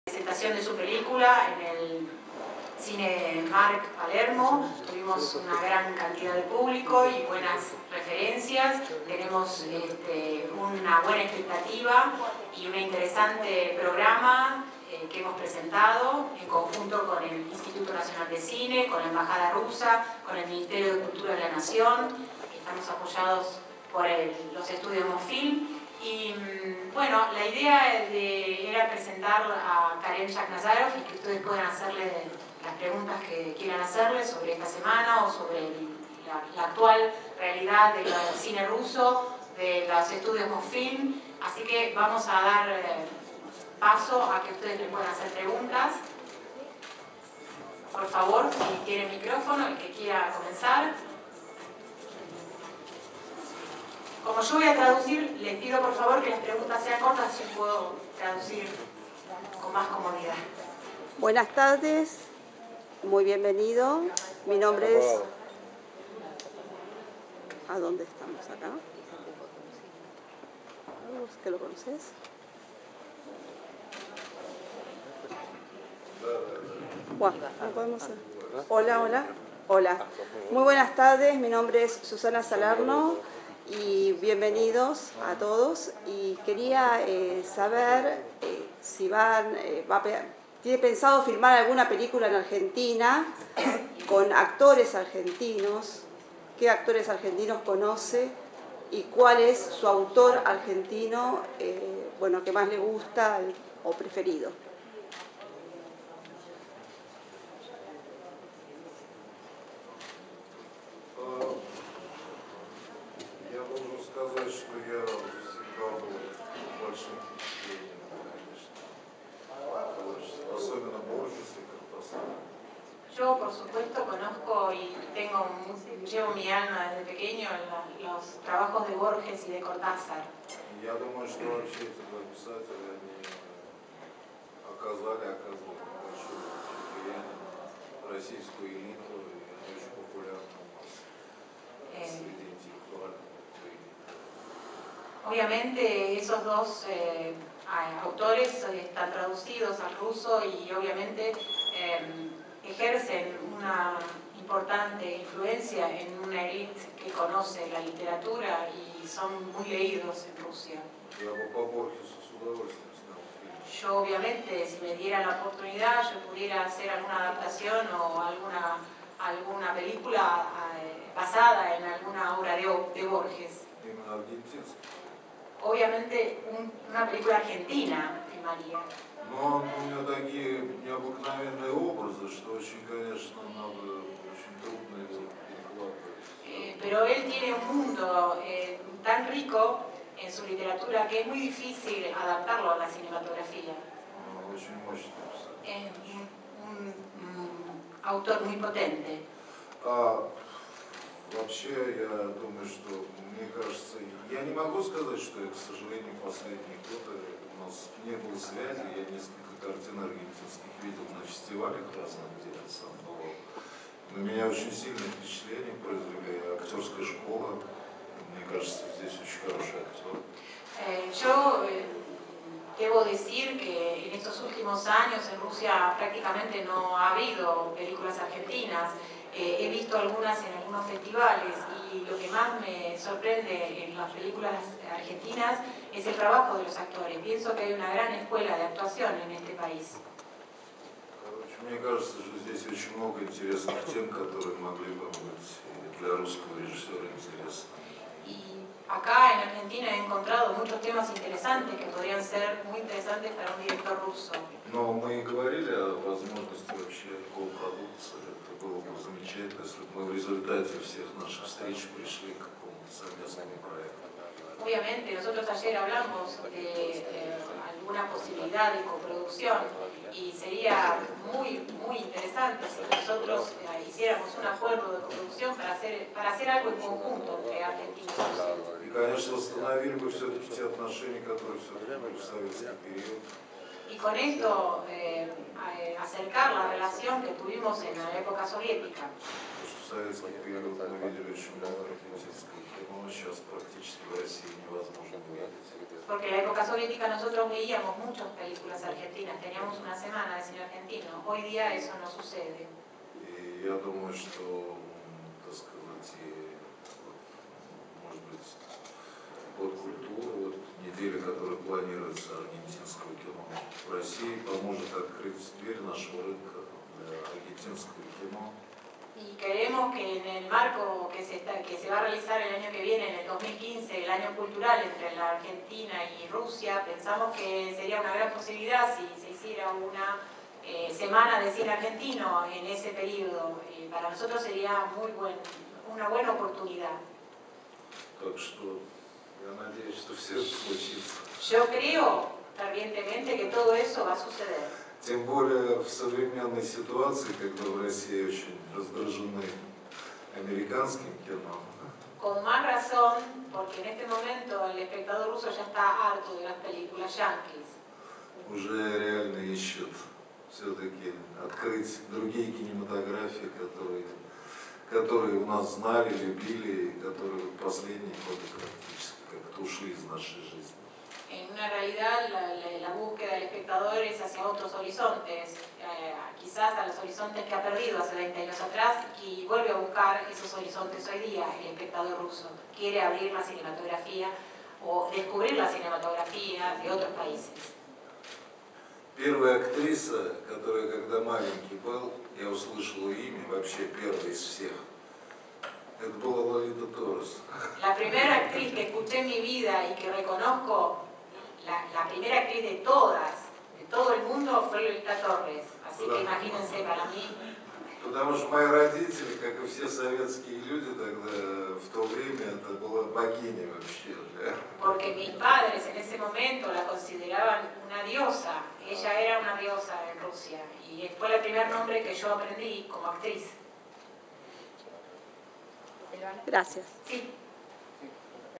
Entrevista al director ruso Karen Shakhnazarov
Se firmó en DAC el Primer acuerdo cinematográfico ARGENTINO-RUSO.
En el marco del actual impulso alcanzado por el desarrollo de las relaciones comerciales y culturales establecidas entre Argentina y Rusia, el viernes 19 de setiembre se firmó en la Casa del Director audiovisual.
Participaron del encuentro celebrado con la asistencia de la prensa en general y miembros de la actividad audiovisual en particular,  la Ministra de Cultura de la Nación Teresa Parodi, el embajador  de la Federación de Rusia en Argentina  Victor Koronelli y Ariel Direse en representación del Instituto nacional de Cine y Artes Audiovisuales INCAA.
La firma del acuerdo estuvo a cargo del Presidente de DAC el realizador argentino Carlos Galettini y el director ruso Karen Shakhnazarov, Presidente de los estudios Mosfilm, quien viajó especialmente a Buenos Aires para rubricar el convenio y presentar su película El Tigre Blanco, nominada al Oscar en 2012 y proyectada ahora en la Semana del Reecuentro con el Cine Ruso. Exhibición totalmente remasterizada de las películas de la  cinematografia rusa que más conmovieron al mundo, organizada y presenta por DAC y auspiciada por el Ministerio de Cultura de la Nación y el INCAA, desde el 18 al 24 de setiembre en los cines Arte Multiplex Belgrano y Cinemark Palermo.

Susana Salerno: ...

Karen Shakhnazarov: ...

Cobertura: Susana Salerno.